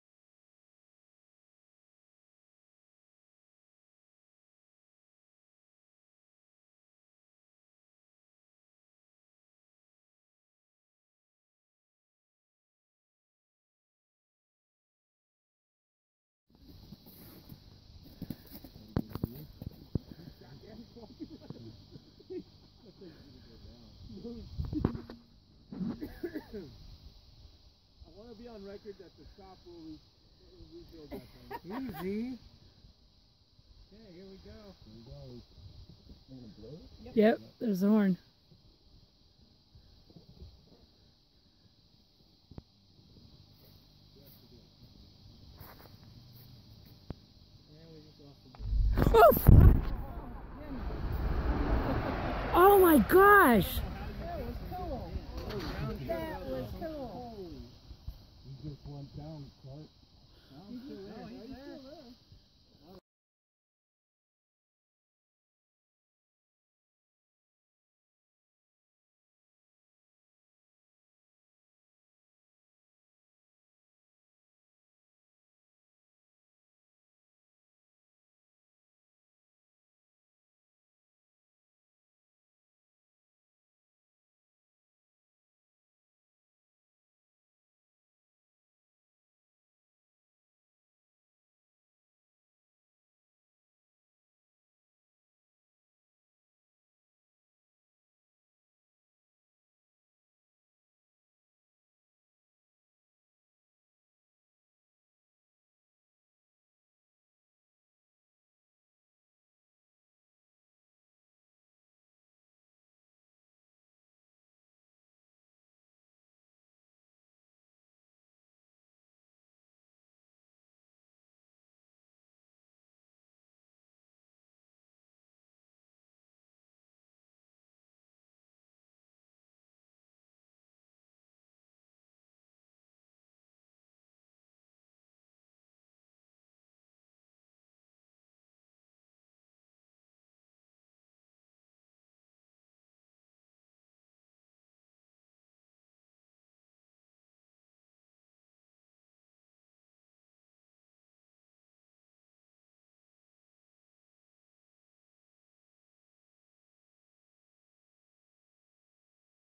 Rock Pit 2 W Sound
Same video as the first but a shorter version with sound added from someone recording from the ground. I just melded the audio to my drone so you can hear the reaction and sound from the blast.